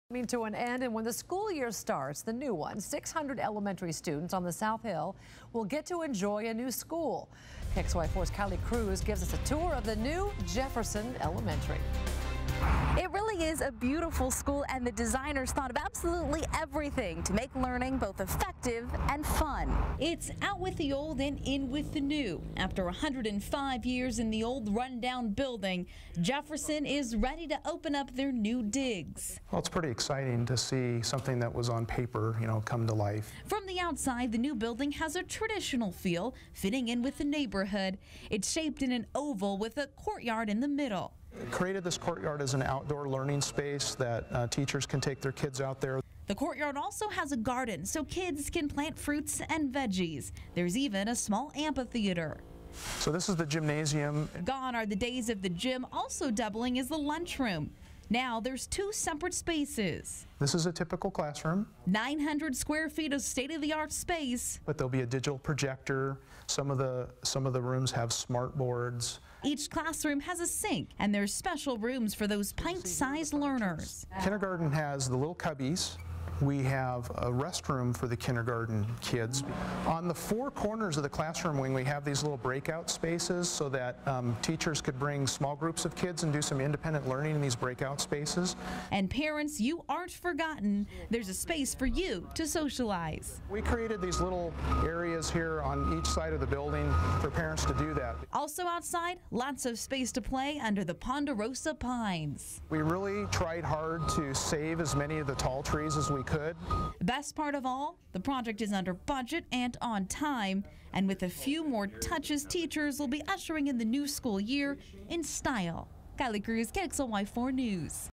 Jefferson Elementary finished on time, under budget
Summer is slowly coming to an end and when the school year starts 600 elementary students on the South Hill are moving into a new school. KXLY4's Kylee Cruz reports.